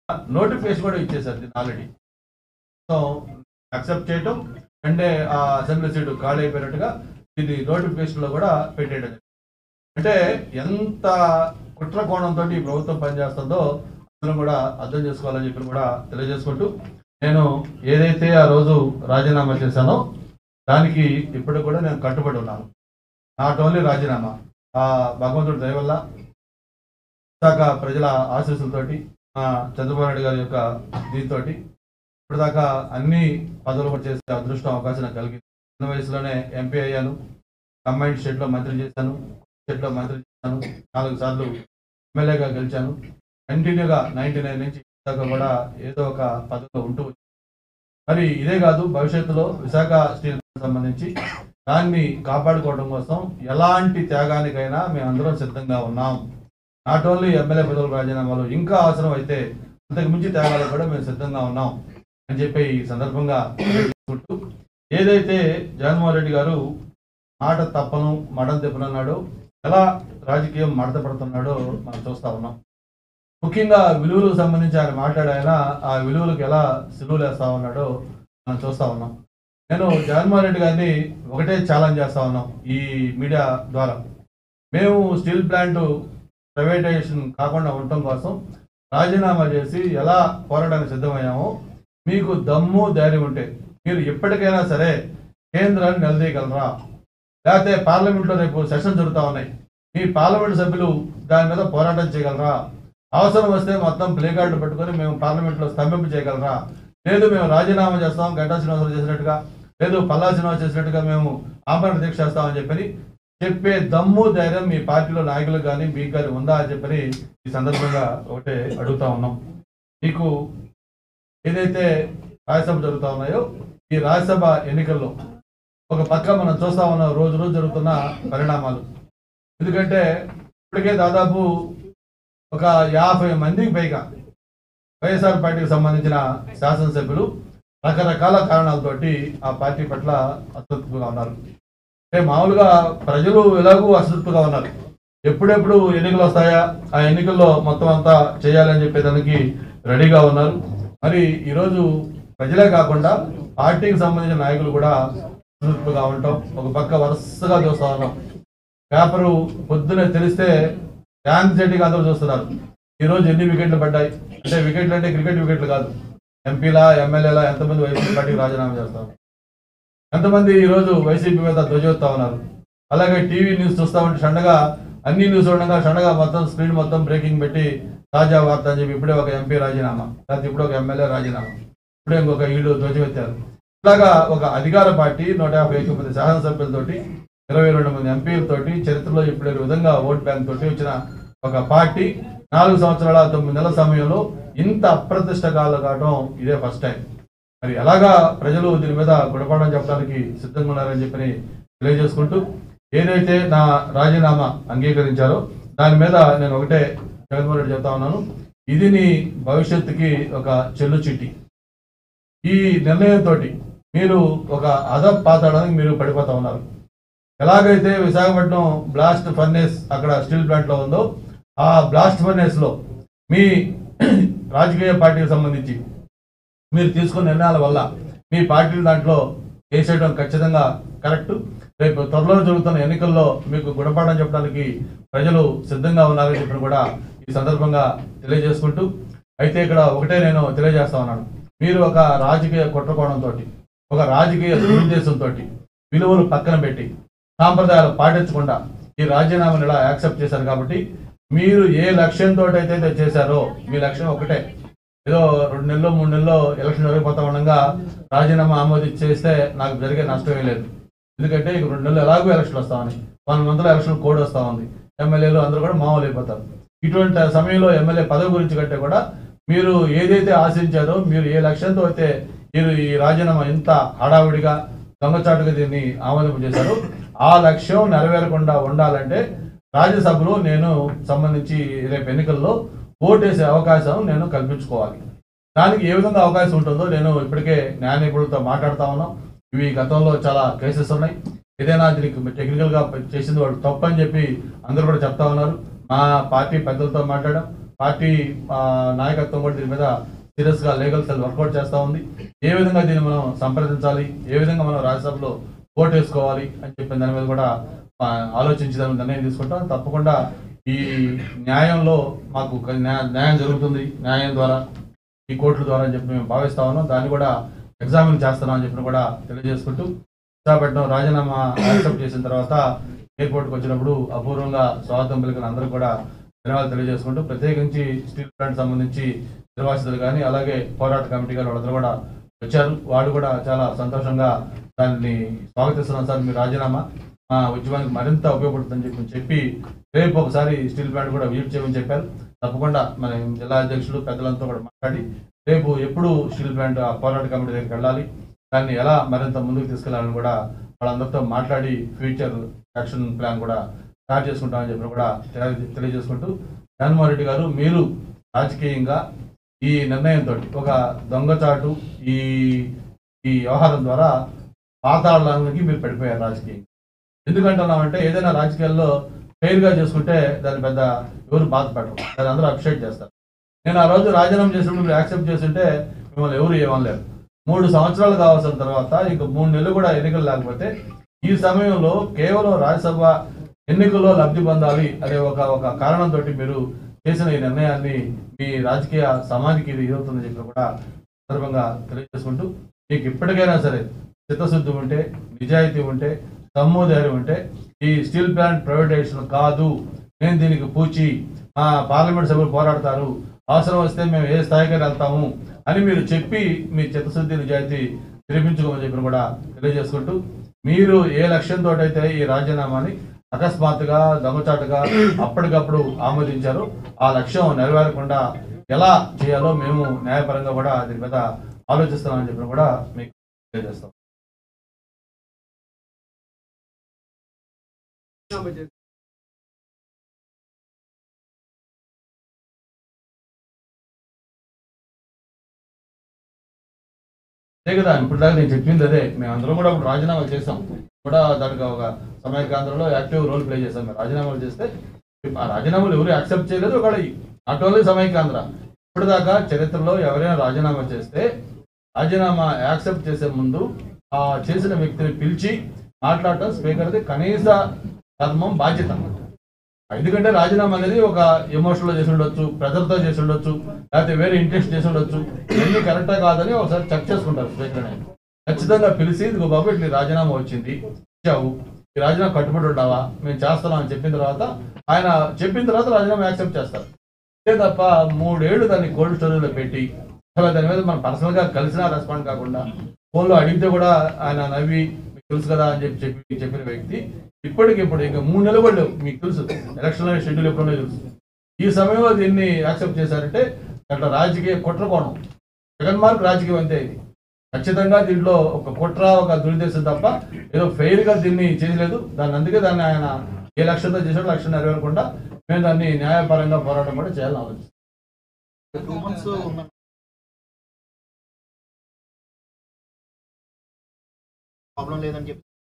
Ganta Srinivasa Rao Says Sorry To Chandrababu Naidu And Joins In Janasena Party | TC Brother
Ganta Srinivasa Rao Says Sorry To Chandrababu Naidu And Joins In Janasena Party